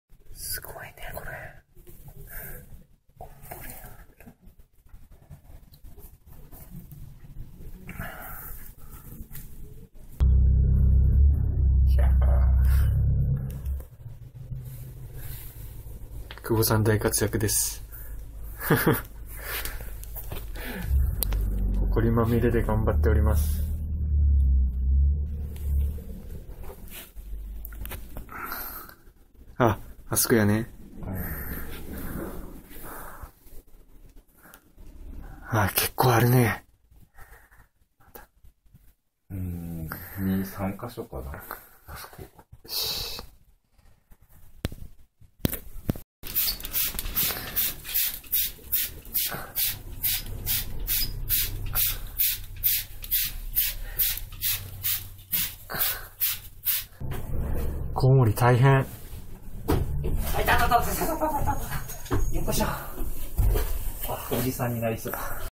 【コウモリ】の進入経路遮断とフンのお掃除/Blocking the 【bat's】 invasion route and cleaning the dung
築３０年のお屋敷の屋根裏にコウモリが住み着いていました。
進入経路を塞ぎ、フンの掃除のした際の１シーンです。
Bats settled in the attic of a 30-year-old mansion.
It is a scene when the entrance route is blocked and the crap is cleaned.